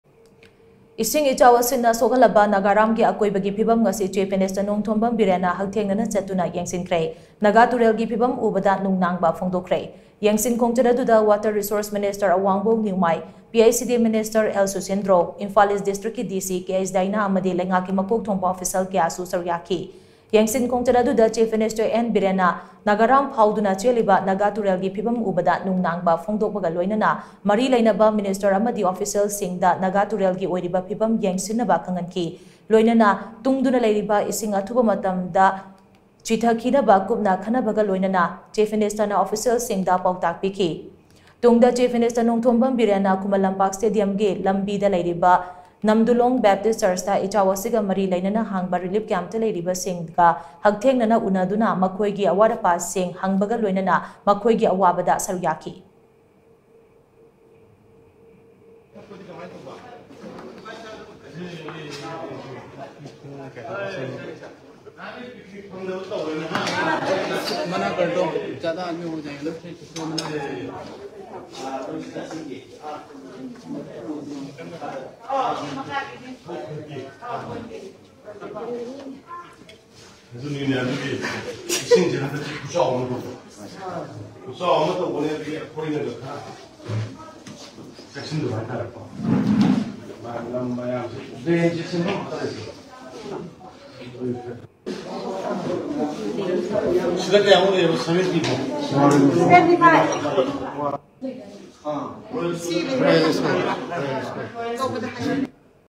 ICHAO NA SOKHALABA NAGARAM CM NA YENGSINKHRE; NAGA TUREL GI PHIVAM UBADA NUNGNANGBA PHONGDOKHRE
Eshing ichao na shok-hallaba Nagaram gi phivam ngasi Chief Minister Nongthombam Biren na hakthengnana chattuna yengshinkhre; Naga turel gi phivam ubada nungnangba phongdokkhre.